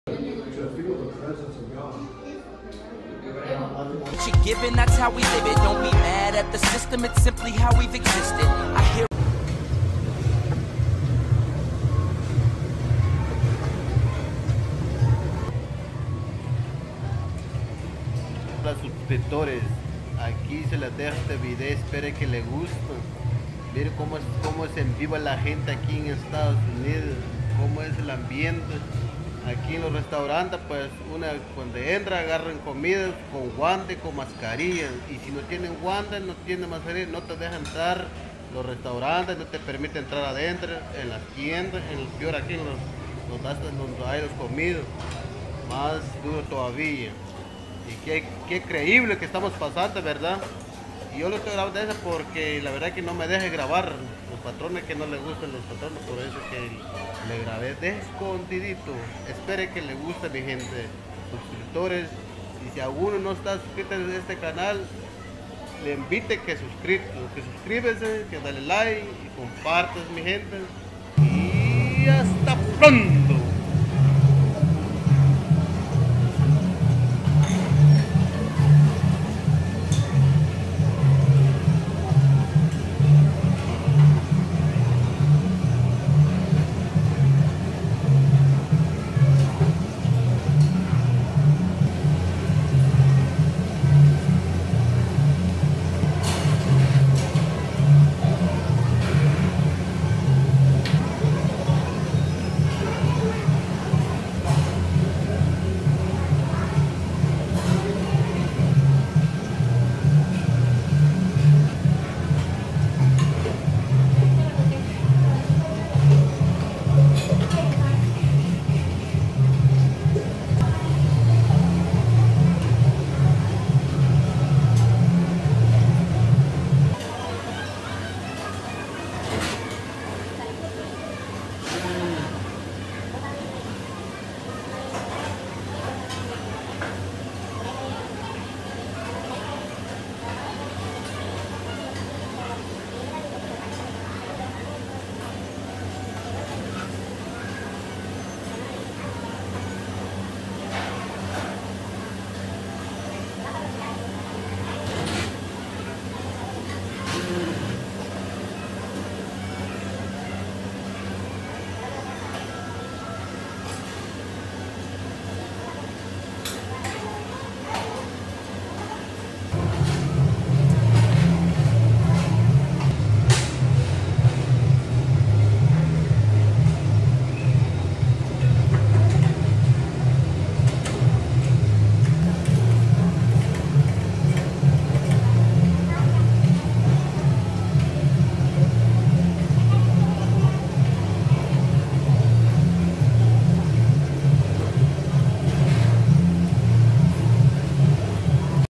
En los restaurante buffet así se mantiene todos los días
Con máscaras y con guantes   Increíbles